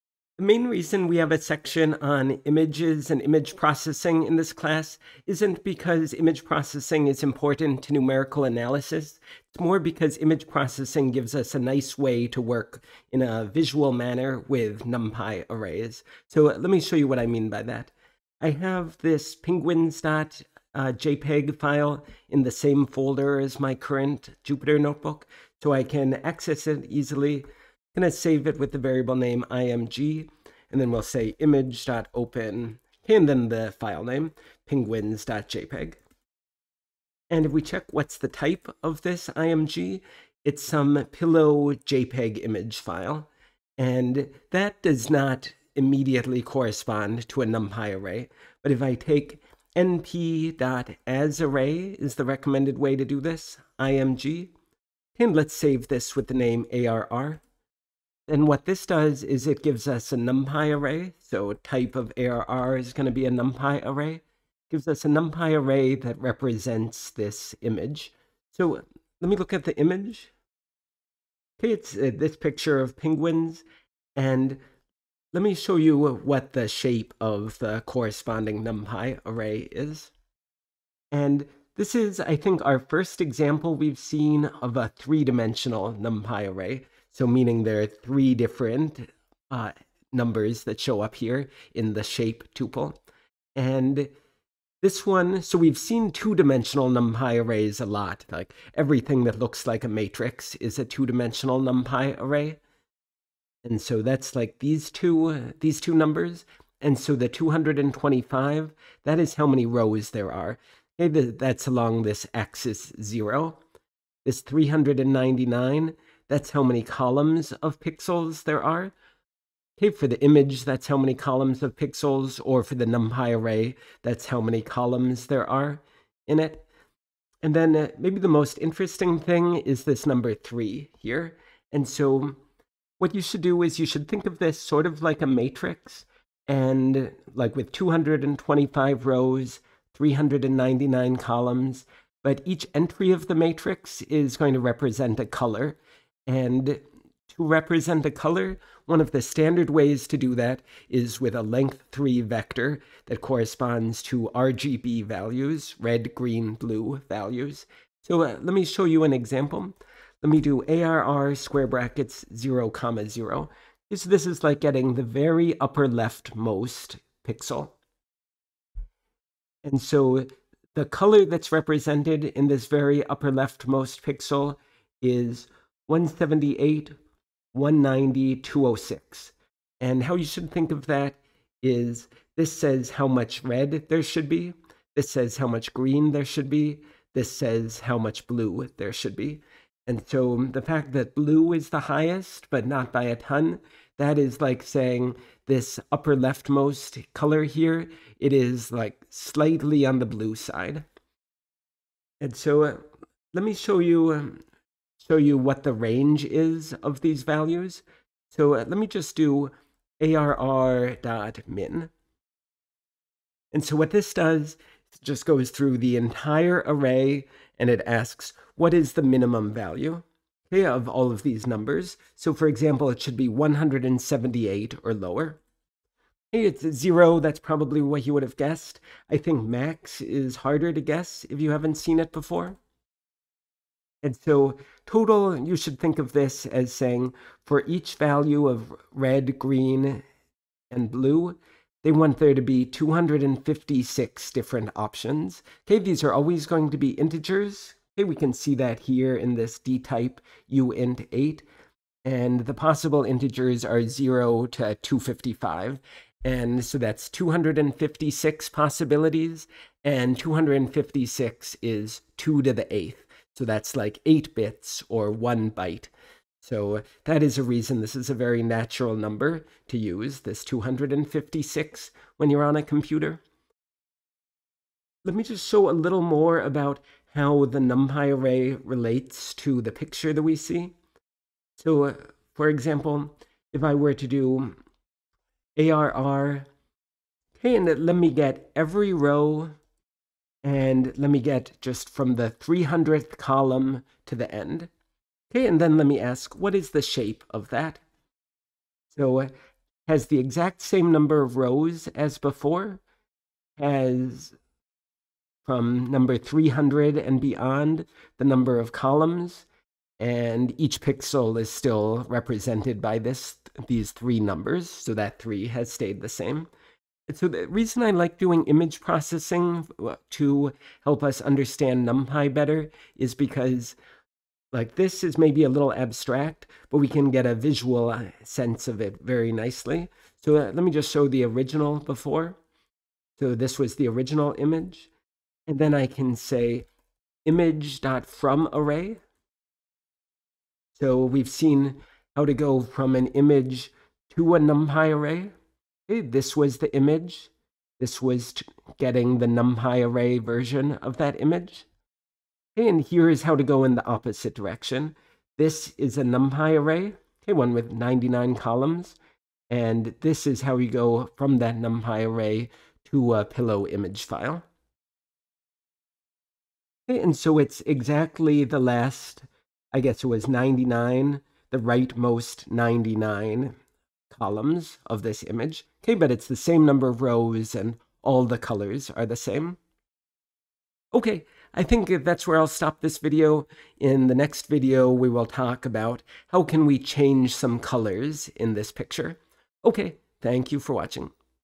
Pillow image processing 2: Images and NumPy arrays
00:00 NumPy array representing an image
2:38 RGB values
4:34 NumPy array to image